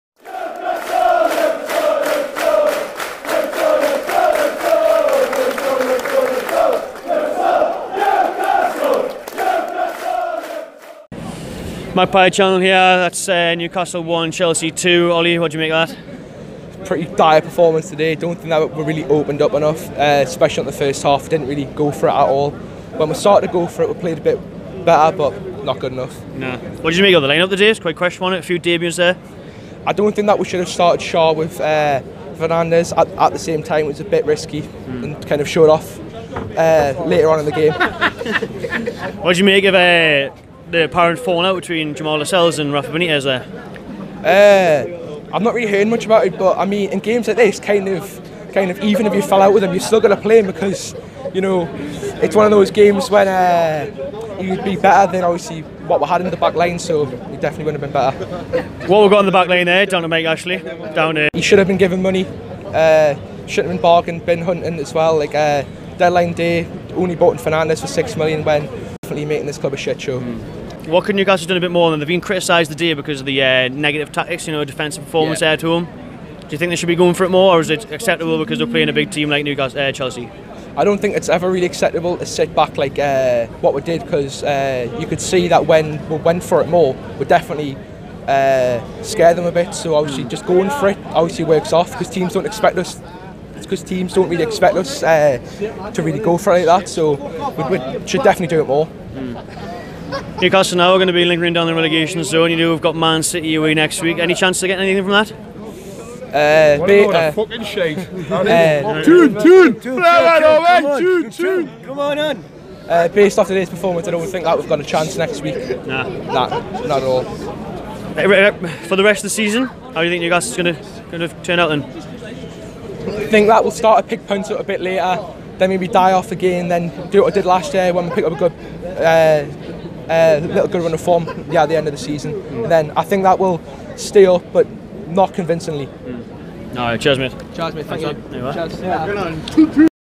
Newcastle 1-2 Chelsea | "Mike Ashley Has Turned NUFC Into A Sh*t Show"
Fan reactions to the game against Chelsea.